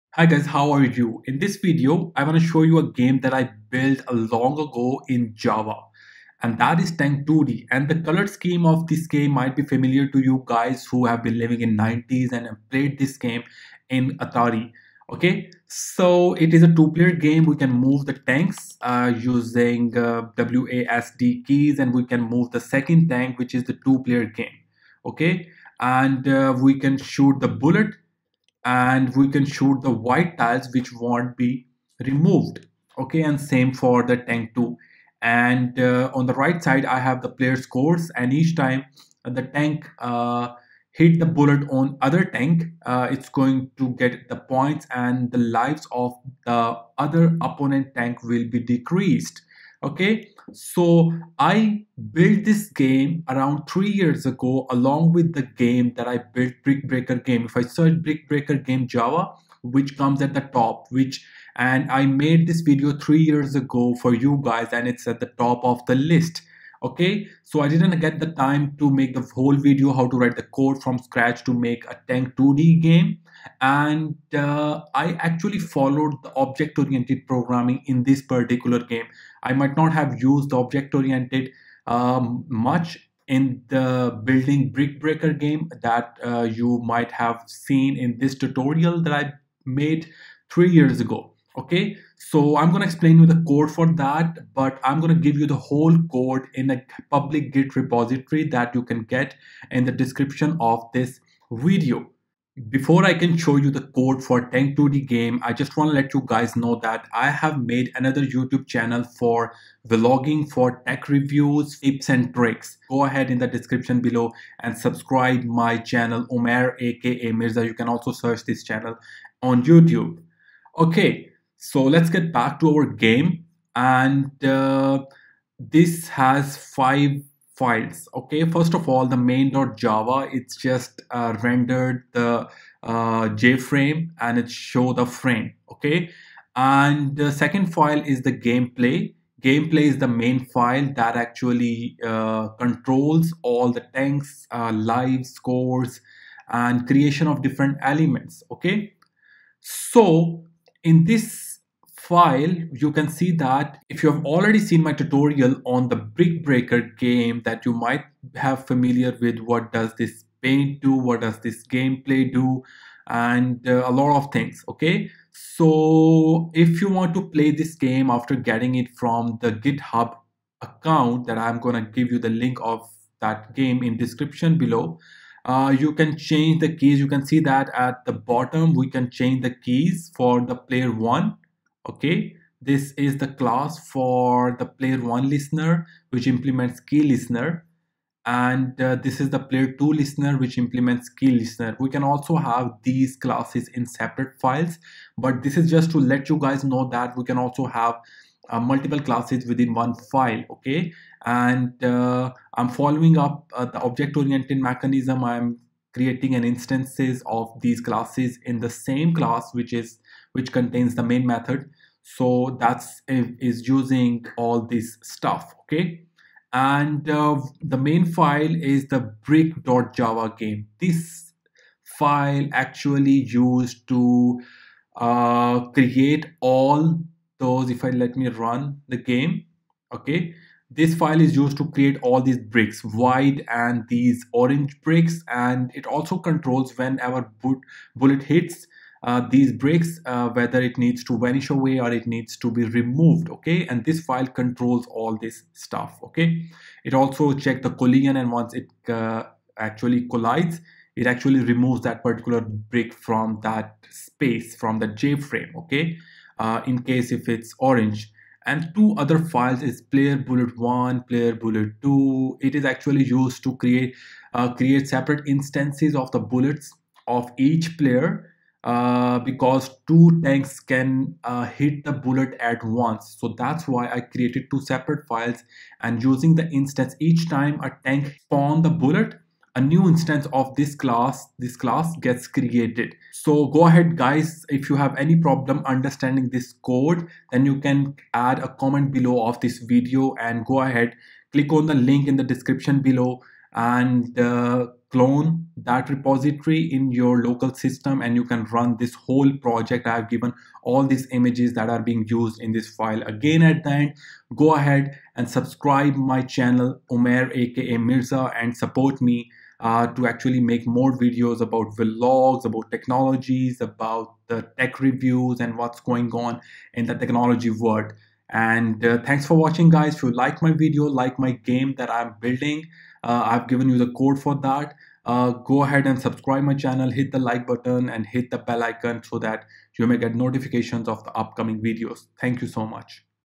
Java Game Programming - Multiplayer Tank 2D Game
Building a multiplayer tank 2D game in Java. Get the code and build this game.

Best Book for Java

Java game programming is really easy to learn. We will develop a brick breaker game with java. We will use JFrame and JPanel for drawing different graphics to make this game work perfectly.